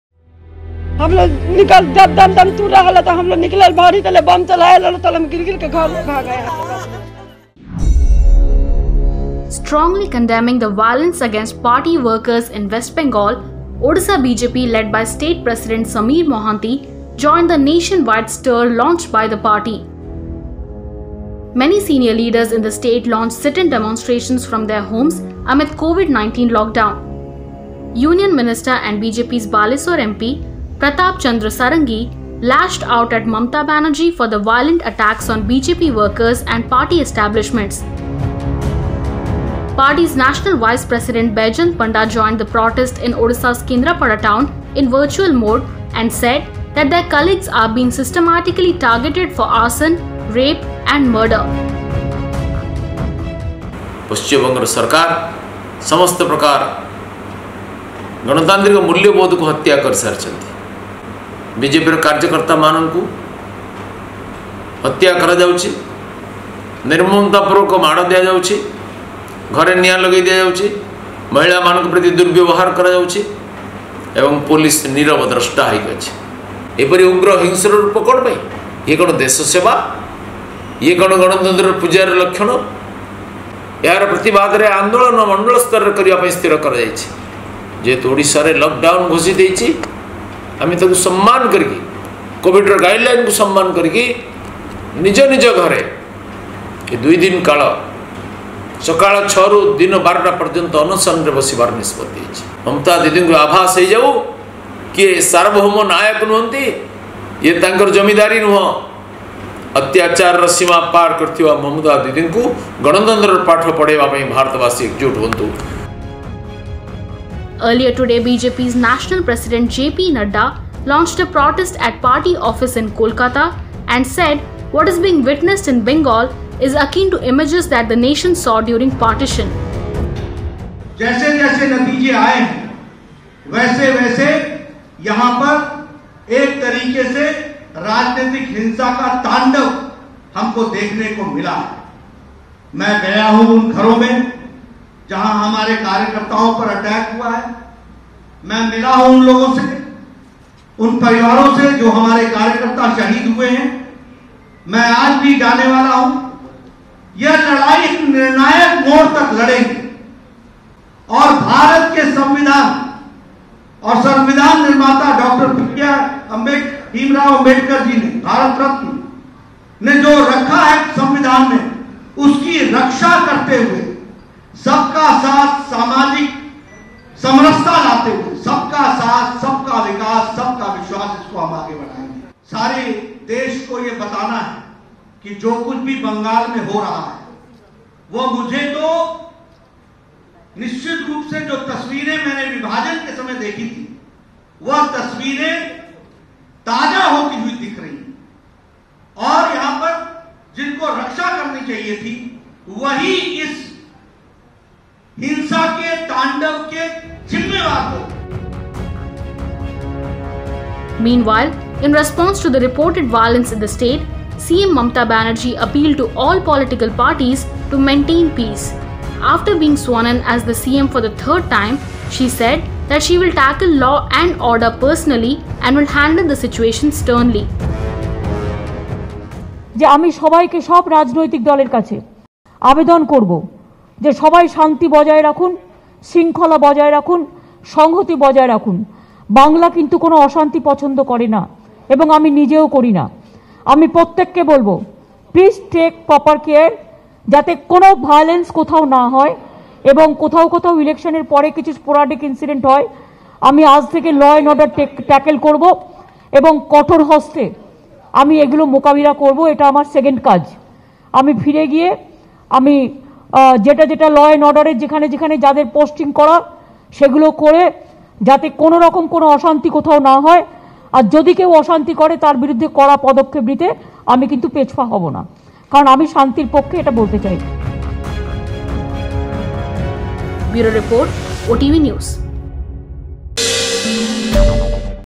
Odisha BJP Stages At-Home Demonstration Protesting Violence Against Party Workers in Bengal
Strongly condemning the violence against party workers in West Bengal, Odisha BJP led by state president Samir Mohanty joined the nation-wide stir launched by the party. Many senior leaders in the State launched sit-in demonstrations from their homes amid COVID19 lockdown. Union Minister and BJP’s Balasore MP, Pratap Chandra Sarangi lashed out at Mamata Banerjee for the violent attacks on BJP workers and party establishments. Party’s National Vice President, Baijayant Panda joined the protest in Odisha's Kendrapada town in virtual mode, and said that their colleagues are being systematically targeted for arson, rape & murder.